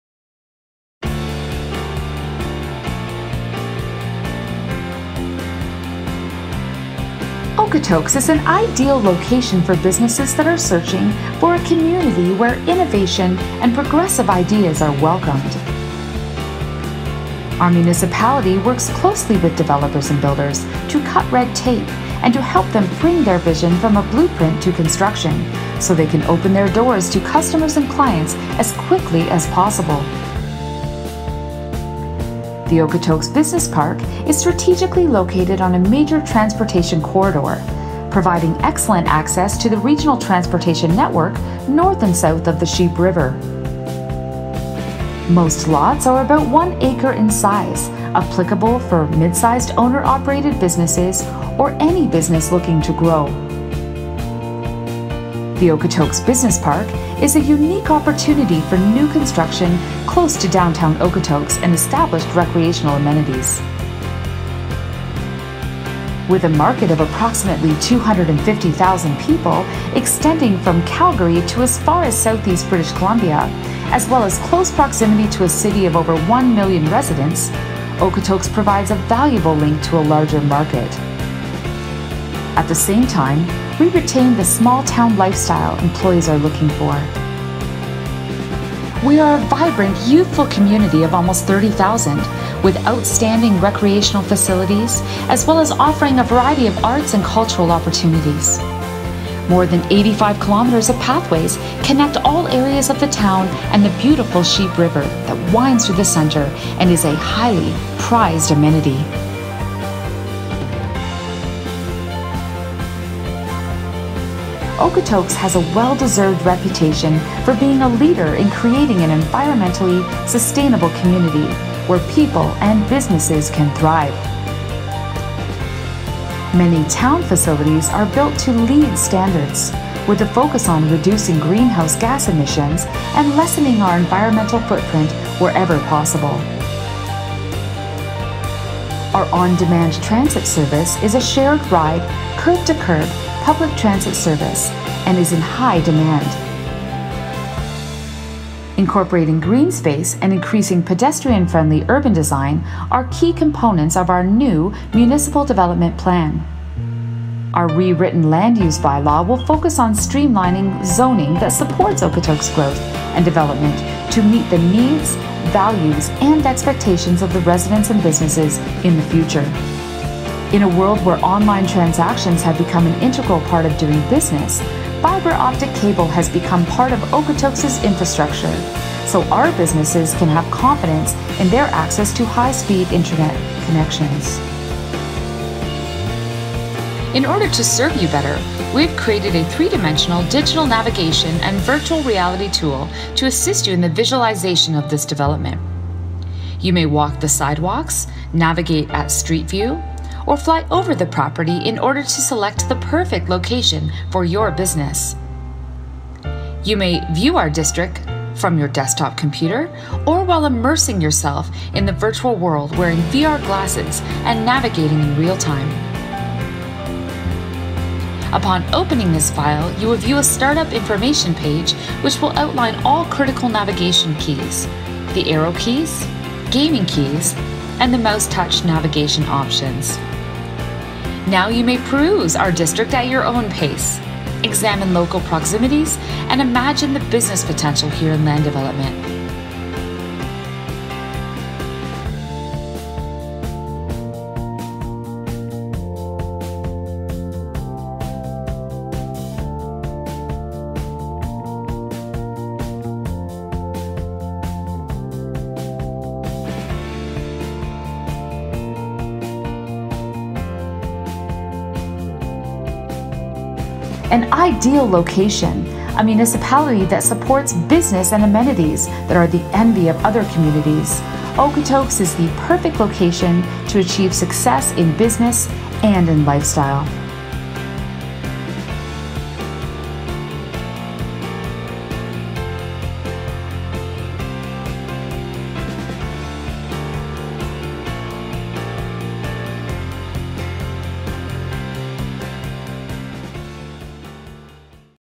Okotoks Business Park Virtual Tour
Take a tour of the potential future in Okotoks industrial area. This animated video provides a vision of how businesses could realize their vision in our community.